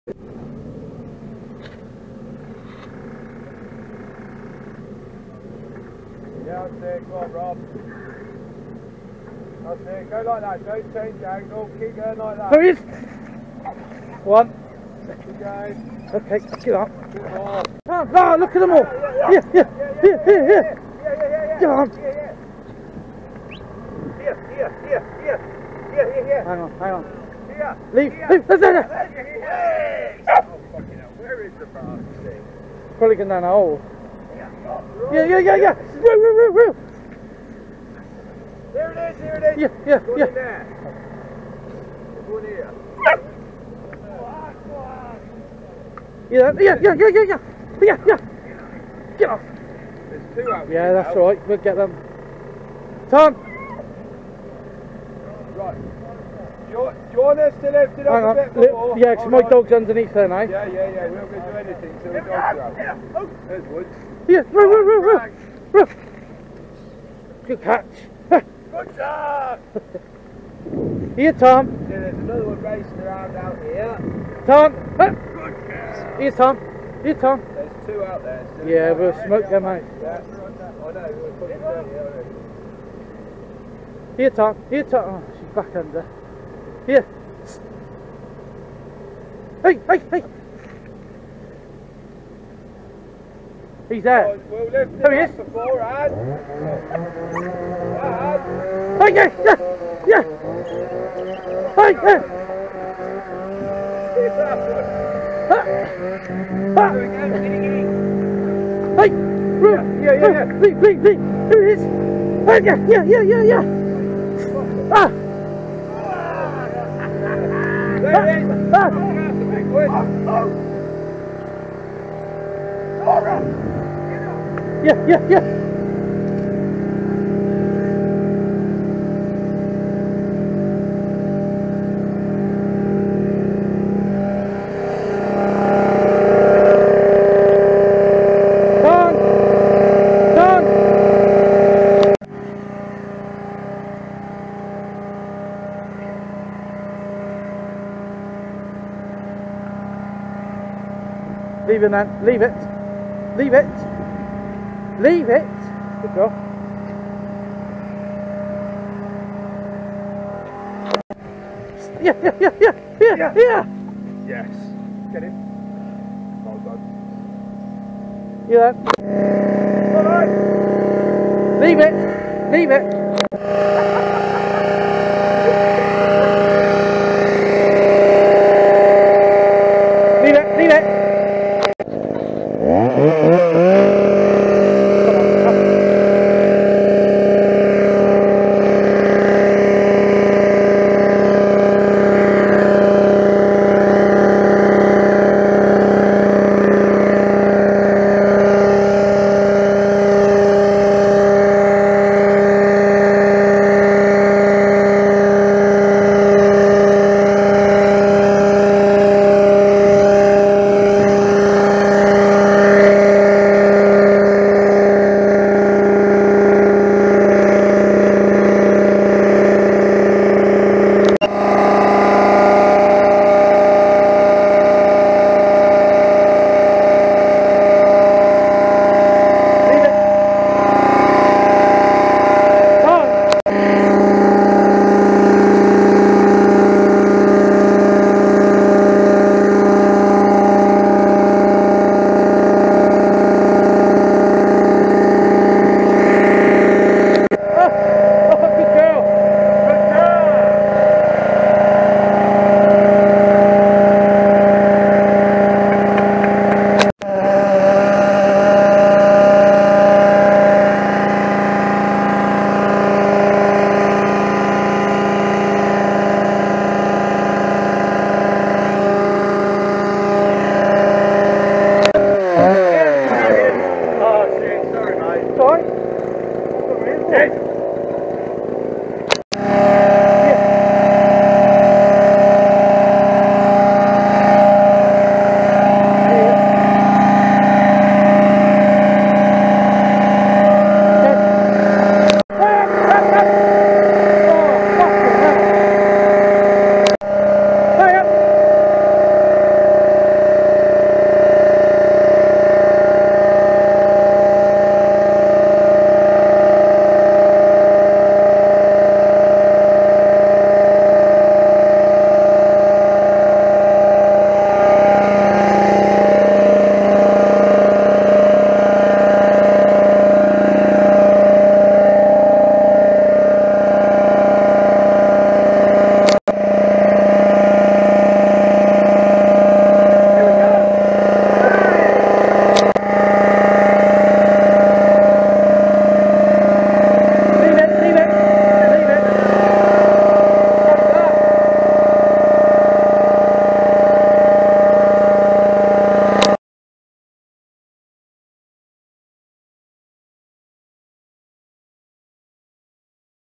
Super Bedlingtons
Border raiders rat pack was invited out, Roo and Tarn didn't let the team down. Great to see some decent working bedingtons